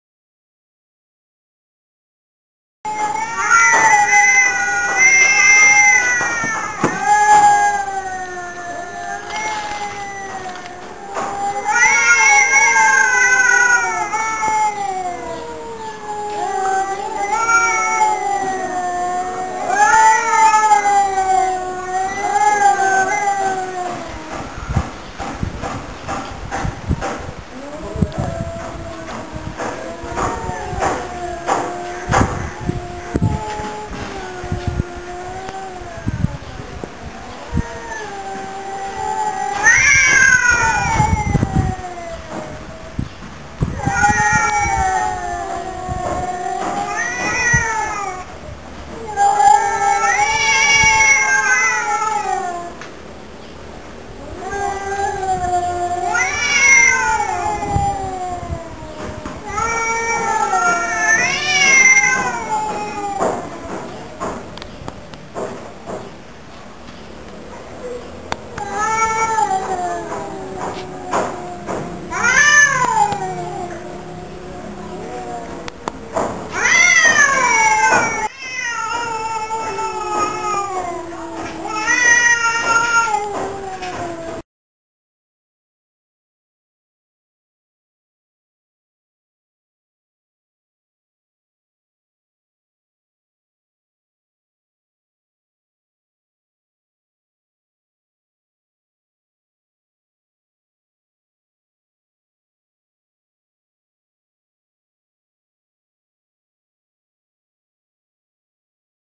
Duet Singing Competition
my 2 cats Mr. Brown and Mr. Black are singing cat songs. Have a look